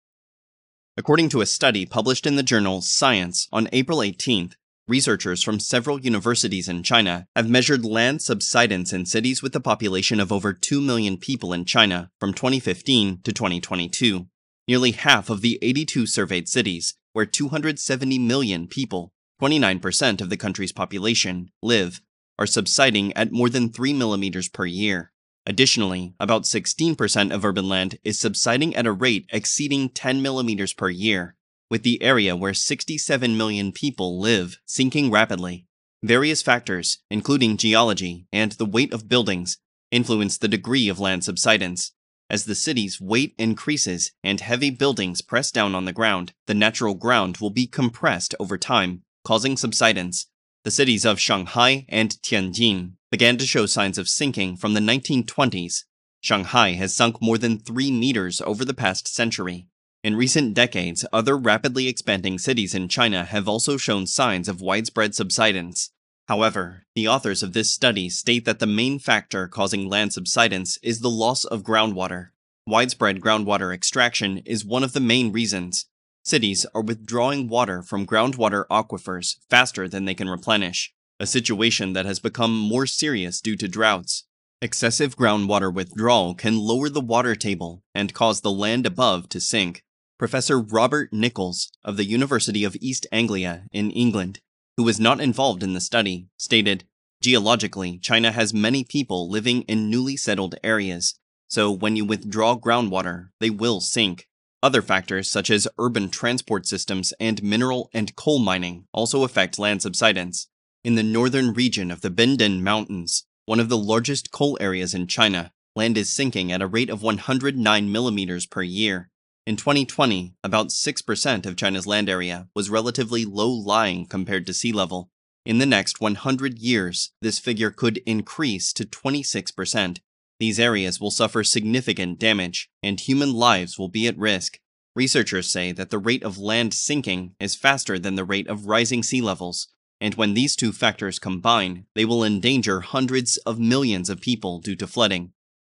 Research: Half of major cities in China are sinking
According to a study published in the Science journal on April 18, researchers from several universities in China have measured land subsidence in cities with a population of over 2 million people in China from 2015 to 2022.

Nearly half of the 82 surveyed cities—where 270 million people (29% of the country's population) live—are subsiding at more than 3 mm per year. Additionally, about 16% of urban land is subsiding at a rate exceeding 10 mm per year, with the area where 67 million people live sinking rapidly.